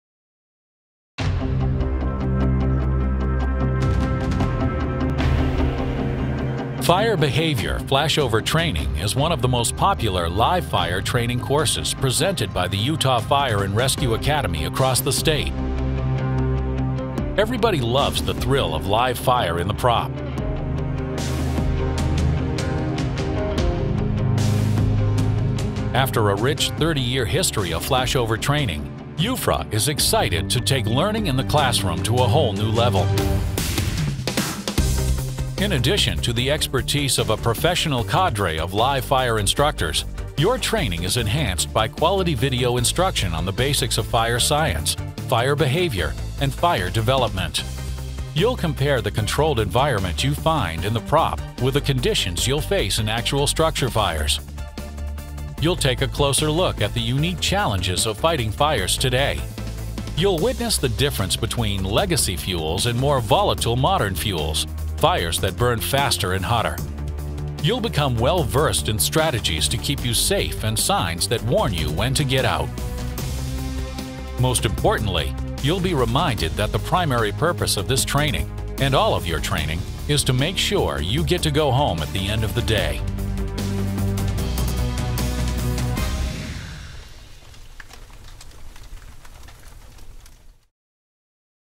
Fire Behavior-Flashover Promo Video
Sneak peek at Utah Fire & Rescue Academy's new Flashover training videos.  Contact the training program manager assigned to your county to schedule a class for your department.